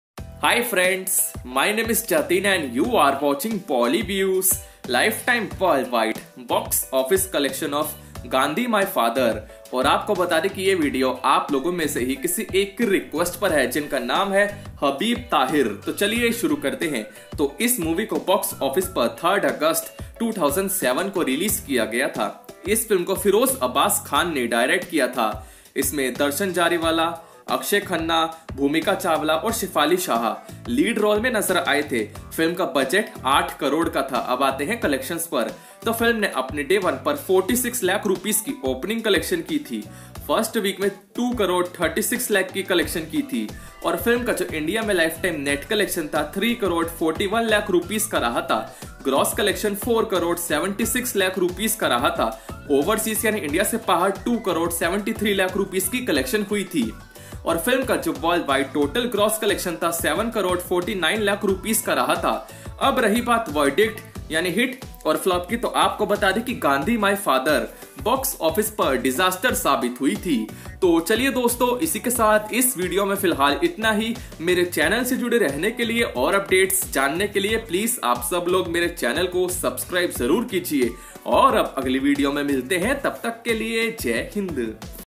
GANDHI MY FATHER 2007 Bollywood Movie LifeTime WorldWide Box Office Collections Verdict Hit or Flop
GANDHI MY FATHER 2007 Bollywood Movie LifeTime WorldWide Box Office Collections
Verdict Hit or Flop
Cast - Akshaye Khanna, Darshan Jariwala, Bhumika Chawla, Shefali Shah